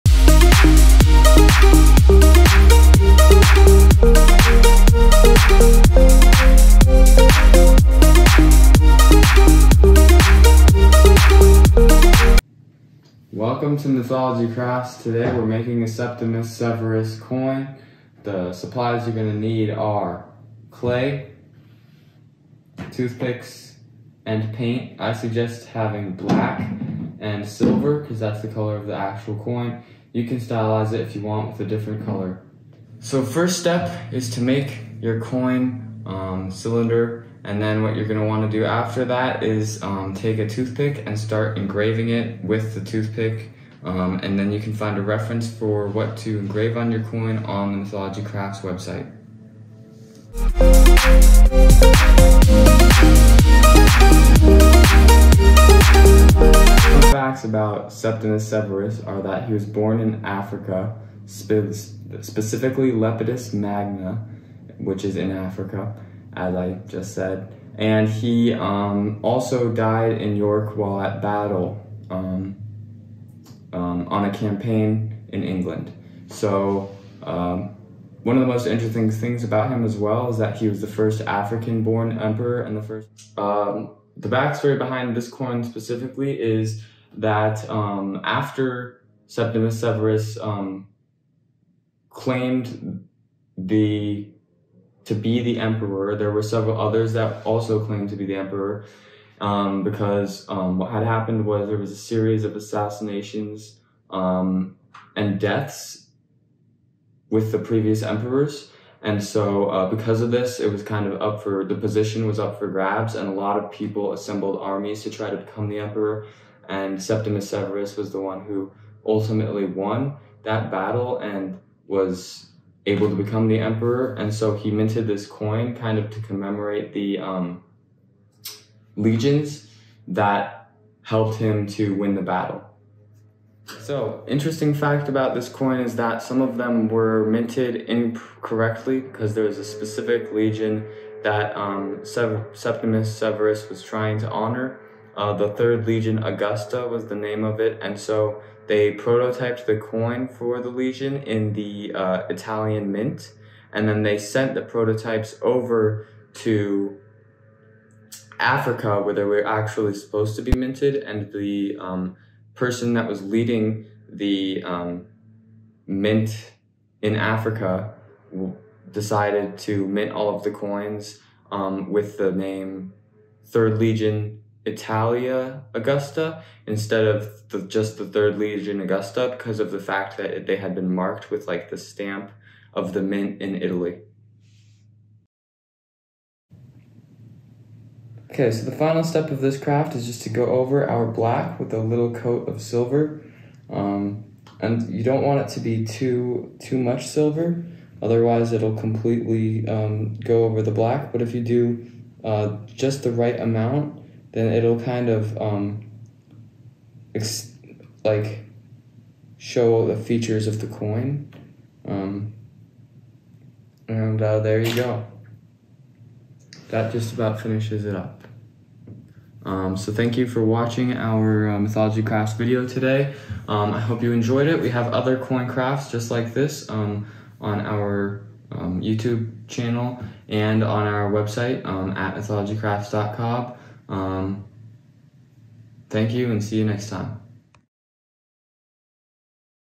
Rome's African Emperor: Septimius Severus & the Legion Augusta Coin
Septimius Severus was Rome's first Emperor born in Africa.  He minted a coin to honor his legions, but it was minted incorrectly for a legion that did not exsist.